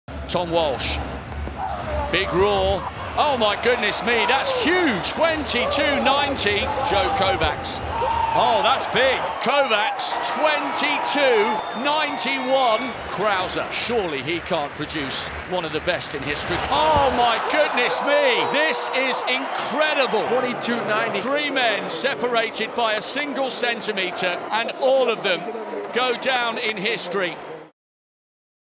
Joe Kovacs Wins Incredible Shot Put Gold | World Athletics Championships 2019 | Doha Moments
It was the greatest men's shot put contest of all time. Just one centimetre separated the medallists as the championship record was broken time and again at the 2019 IAAF World Athletics Championships in Doha. Joe Kovacs shocked the competition to win gold with a monster throw of 22.91m, ahead of pre-tournament favourites Ryan Crouser and Tom Walsh, who both threw 22.90m.